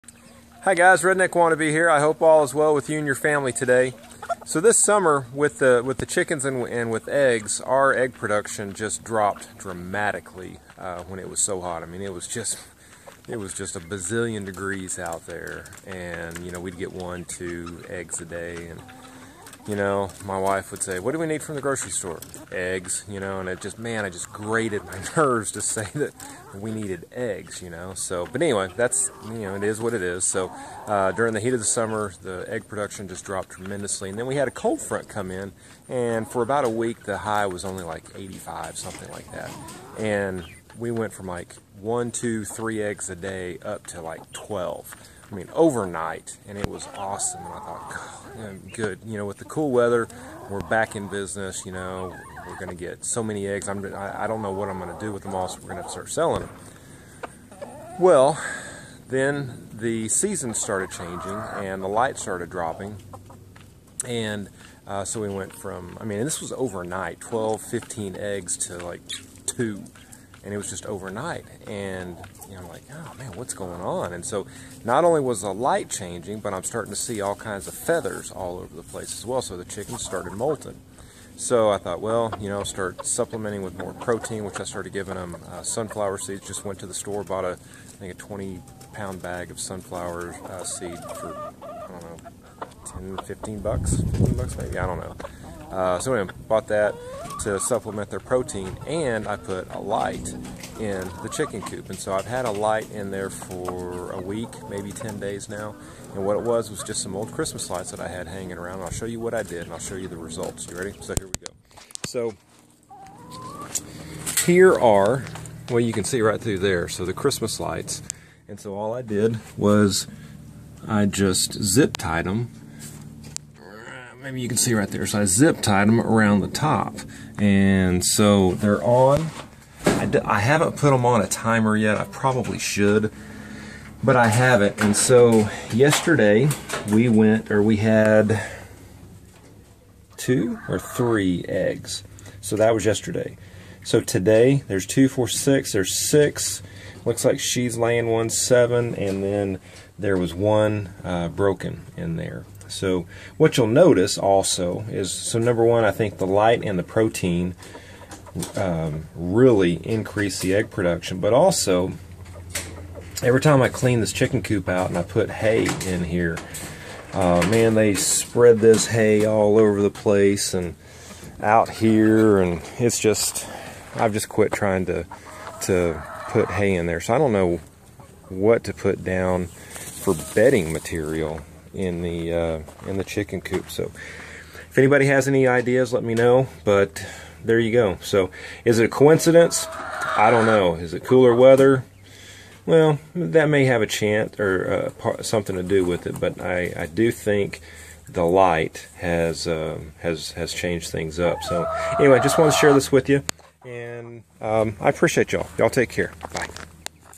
Artificial light and egg production
I filmed this video yesterday and today we had 12 eggs.  Hopefully, the girls are back